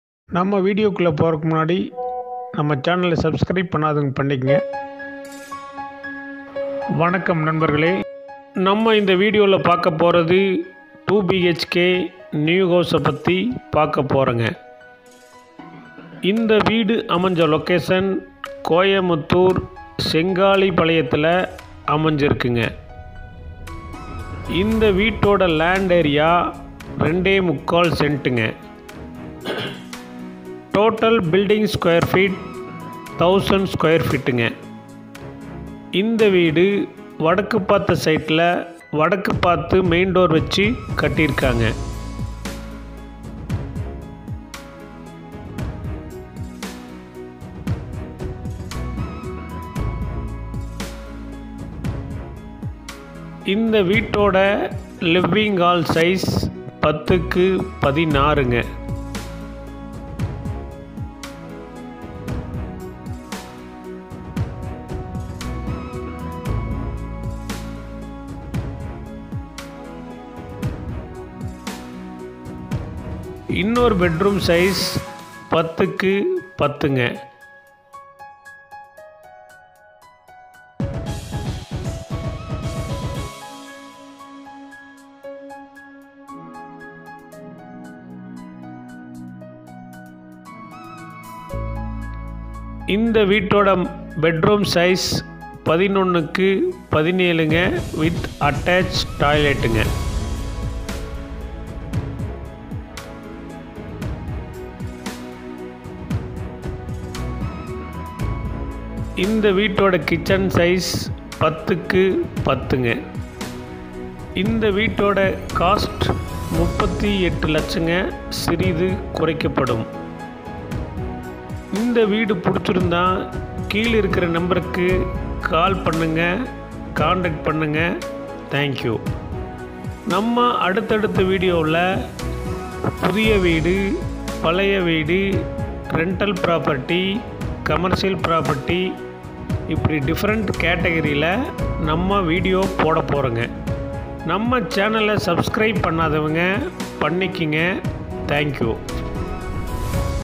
99)புதிய வீடு விற்பனைக்கு |செங்காலிபாளையம் New House for sale Coimbatore I Sengalipalayam📞
North Facing 2 BHK New House for Sale In Coimbatore  Idigarai Sengalipalayam

 வடக்கு பார்த்த 2 BHK புதிய வீடு விற்பனைக்கு கோவை இடிகரை செங்காலிபாளையம்

👉 coimbatoreclassicproperties

Total House Price Including Land is 38 Lakhs
(negotiable)

Location
Coimbatore Thudiyalur  Idigarai Sengalipalayam

Land Details
Site - 2.75 Cent
Site Facing - North Facing

-House Details --

Built up - 1000 Sq fit
Portico - 11*14
Hall - 16*10
Pooja room - 4*5
Kitchen & Dinning - 10*10
Master Bedroom - 10*16 ( Attached Bathroom )
Bedroom - 10*10 (Attached Bathroom )

0